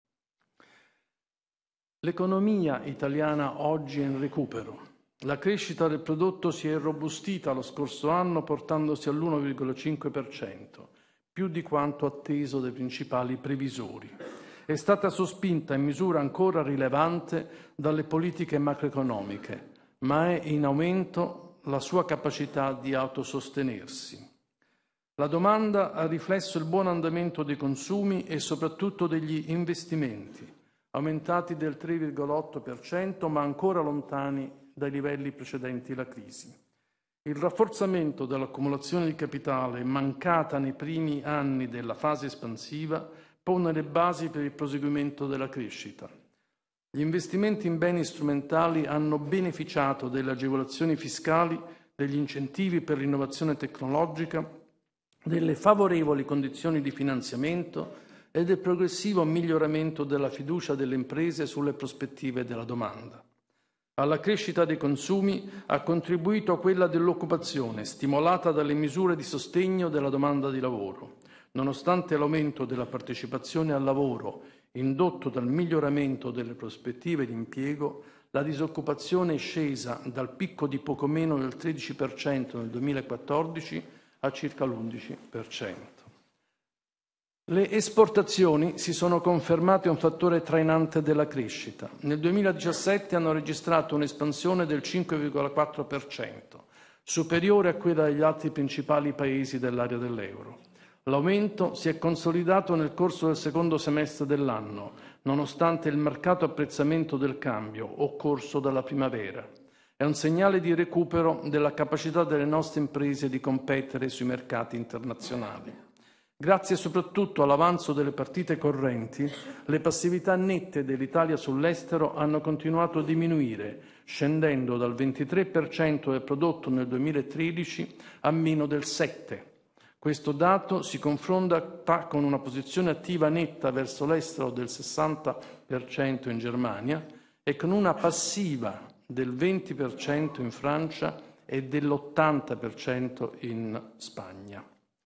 Visco: "L'economia italiana è in recupero"
(Agenzia Vista) - Roma, 29 Maggio 2018 - Il Governatore della Banca d'Italia, Ignazio Visco, nelle sue considerazioni finali della relazione annuale di Bankitalia. "Per ridurre il debito non vi sono scorciatoie. Gran parte del risparmio finanziario accumulato dagli italiani trova corrispondenza, diretta o indiretta, nei 2.300 miliardi del nostro debito pubblico. Se venisse messo a repentaglio il valore della loro ricchezza reagirebbero fuggendo, cercando altrove riparo. E gli investitori stranieri sarebbero più rapidi. La crisi finanziaria che ne conseguirebbe farebbe fare al nostro Paese molti passi indietro. Macchierebbe in modo indelebile la reputazione dell'Italia nel mondo".